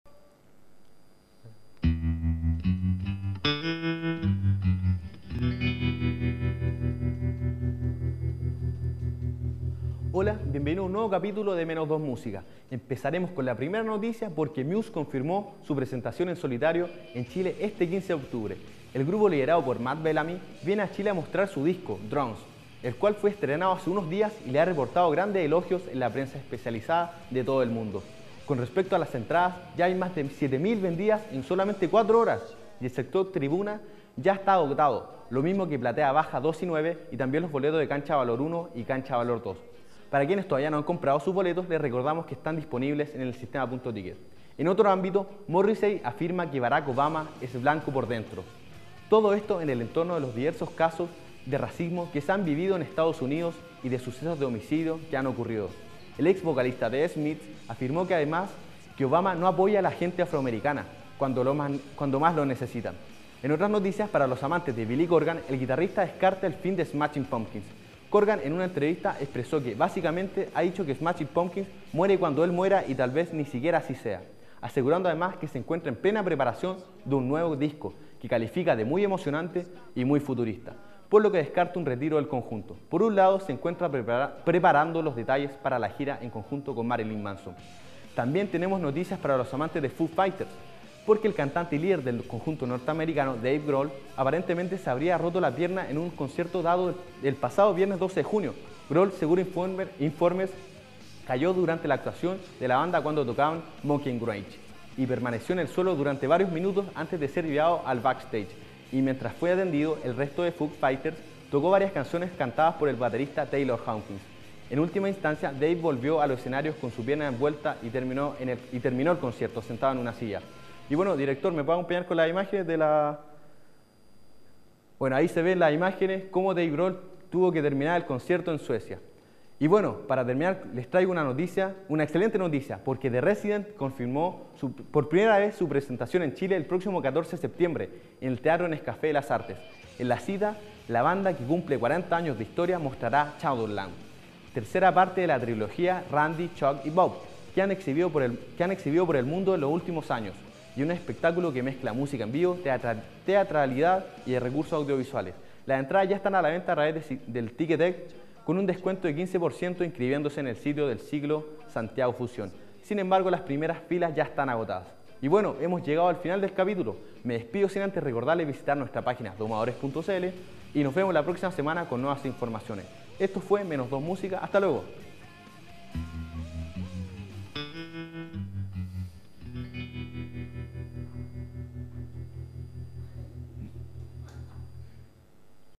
Menos 2 música | Muse confirma su presentación en solitario en nuestro país
En otras noticias, Morrisey da declaraciones racistas, Billy Corgan descarta el fin de Smashing Pumpkins, Dave Grohl sufre accidente en concierto y The Residents por primera vez en Chile.